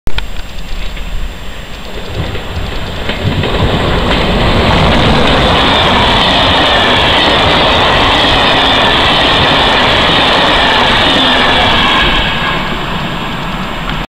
La BB 7215 à St Pé de Bigorre.
Locomotive BB 7215 avec livrée "en voyage" tractant un intercités Toulouse - Bayonne à St Pé de Bigorre sous la neige.
05/12/2012
Film de Christian Fincato